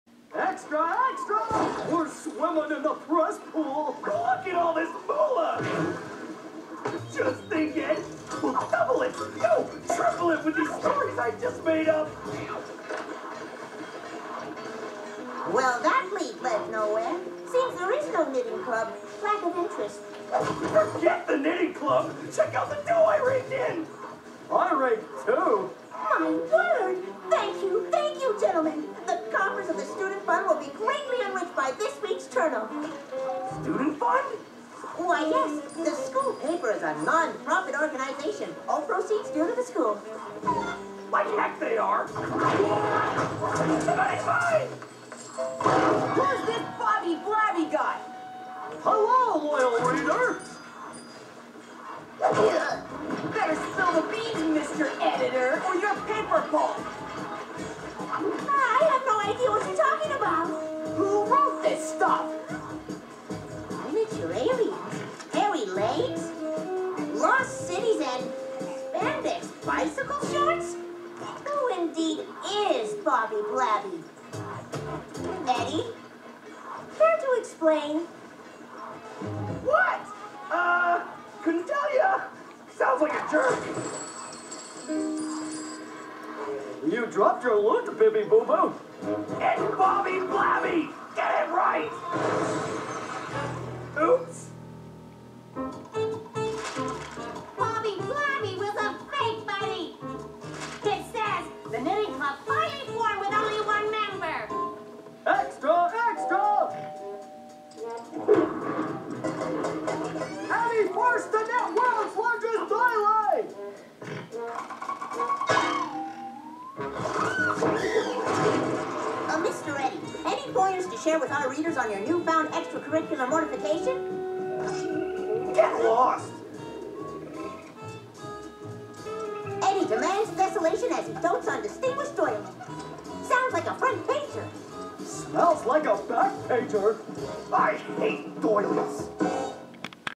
Who is "Bobby Blabby"?
Eddy get's exposed for tricking everyone for through the school paper he is force to join the knitting club.